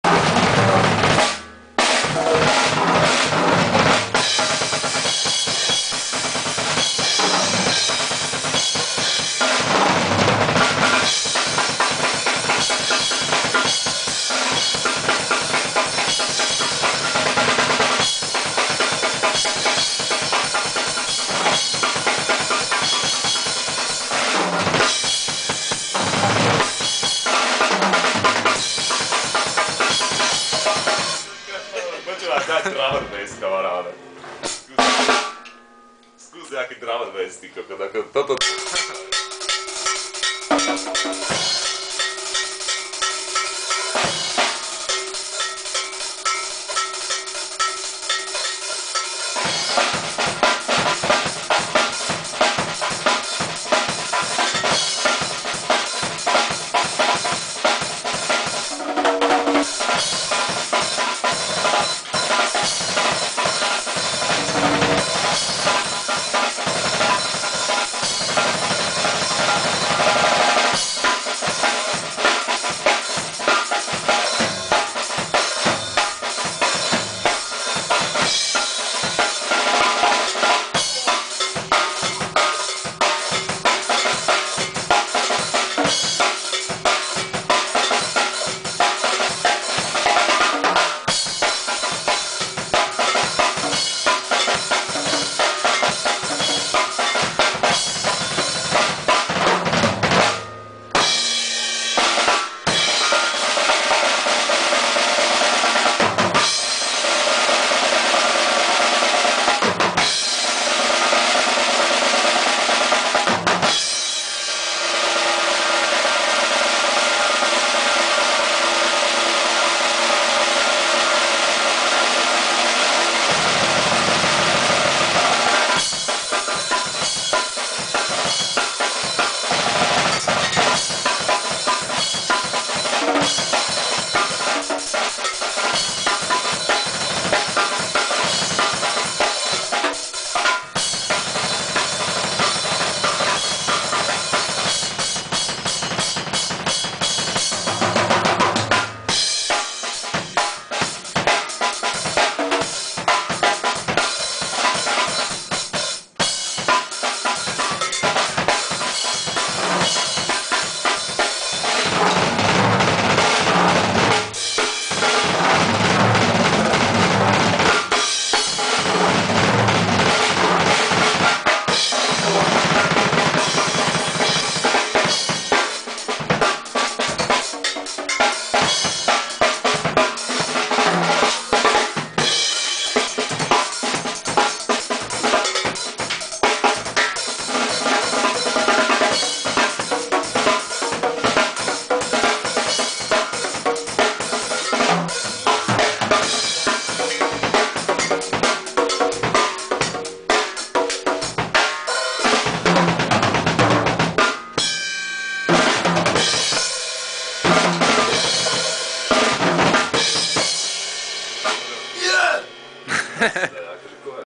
Drum'n'Bass, drum solo Laco Kovac aka Energy-X
First metal and later Drum & bass. Enjoy